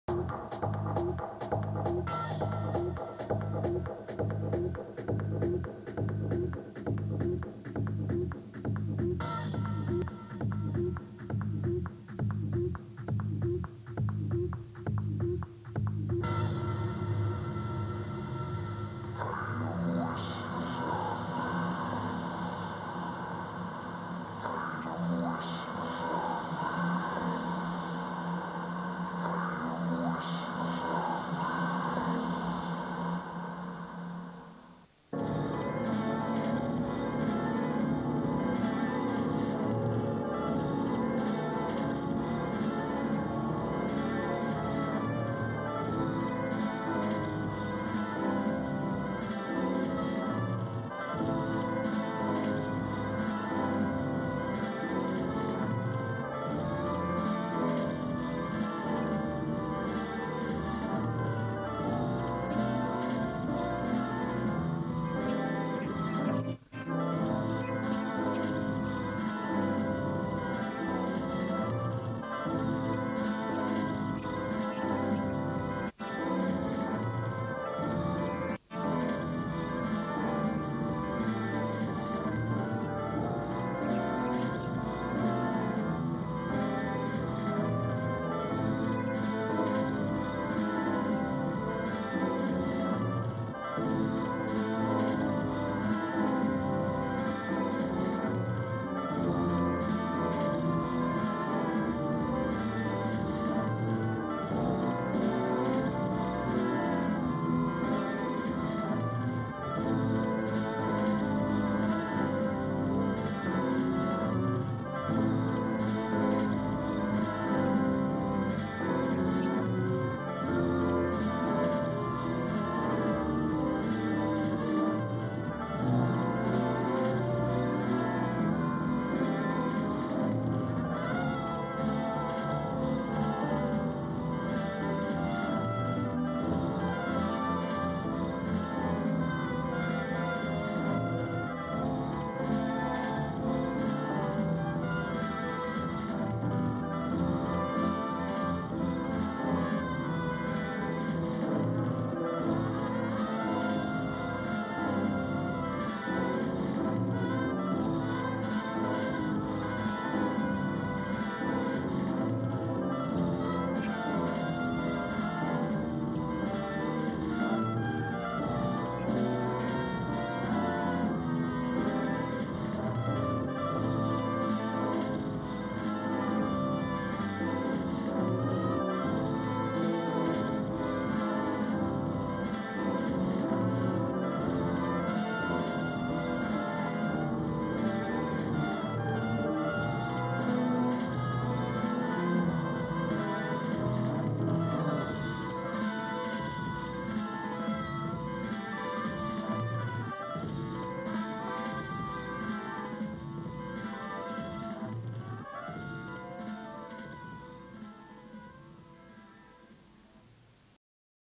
Weird Music
Some weird music/scenes from a channel 4 TV series from the 1990's.